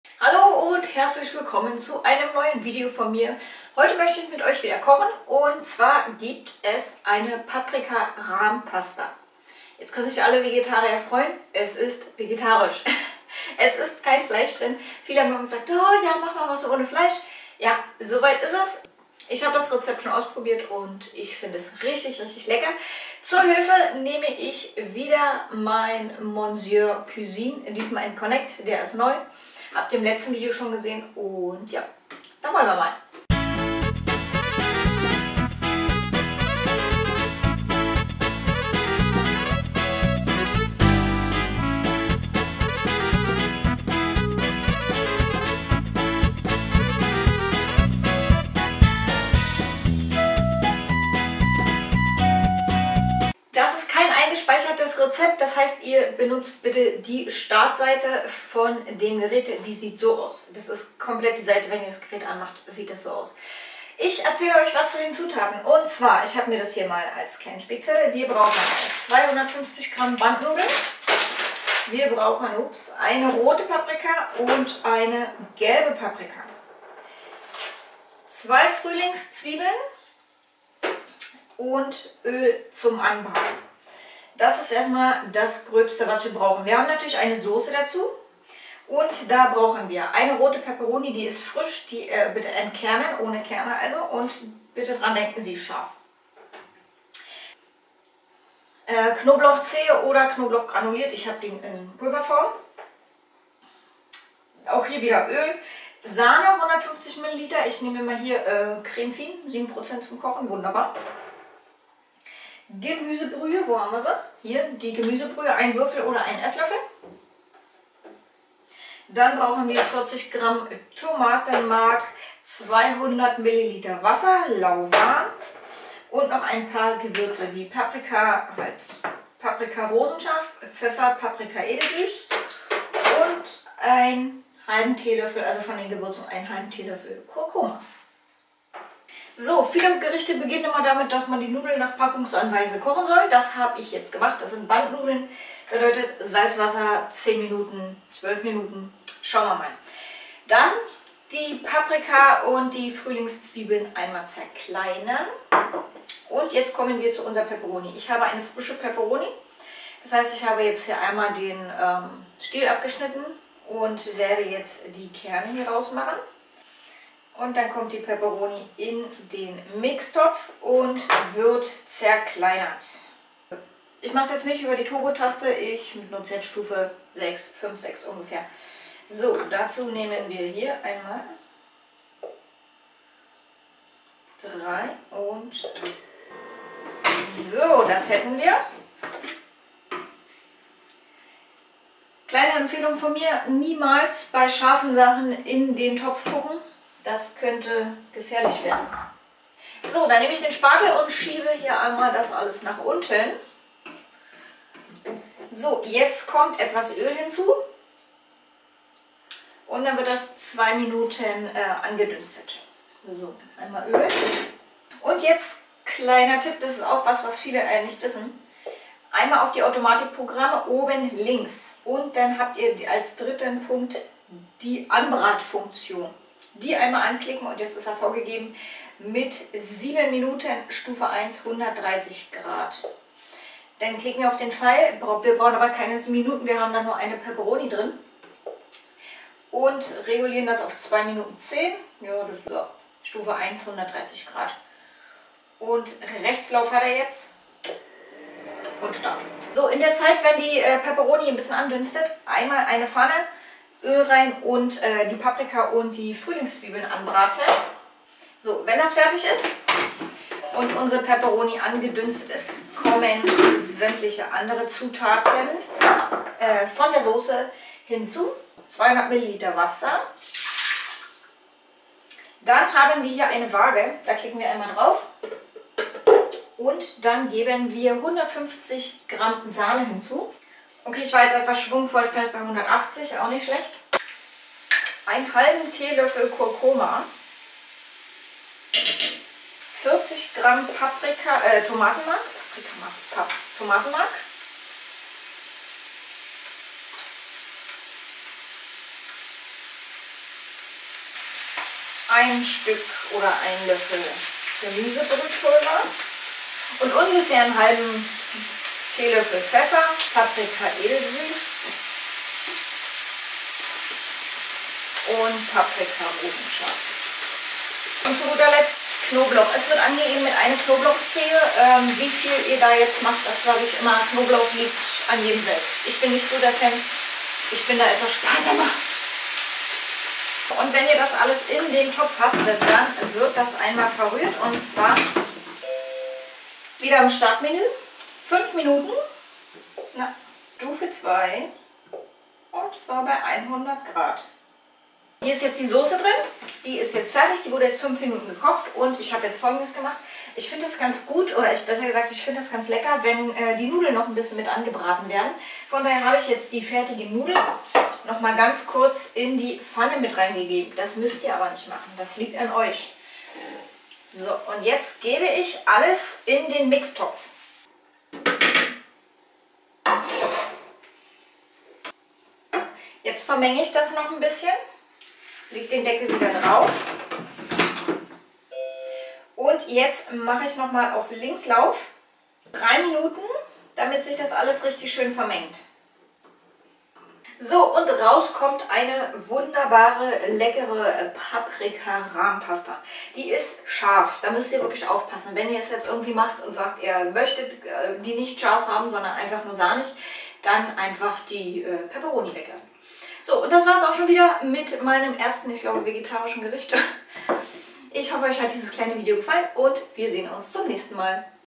Paprika-Rahm-Pasta | Monsieur Cuisine Connect
Heute möchte ich mit euch eine ganz einfache Pasta machen. Die Paprika-Rahm-Pasta
Zutaten:
250g Bandnudeln
1rote und 1 gelbe Paprika
2 Frühlingszwiebeln
150ml Sahne
ÖL
1 rote Peperoni
Knoblauch
200g Wasser
1 EL Gemüsebrühpulver ( 1 Würfel)
1/2 TL: Kurkuma
Pfeffer
Paprika scharf und edelsüß
40g Tomatenmark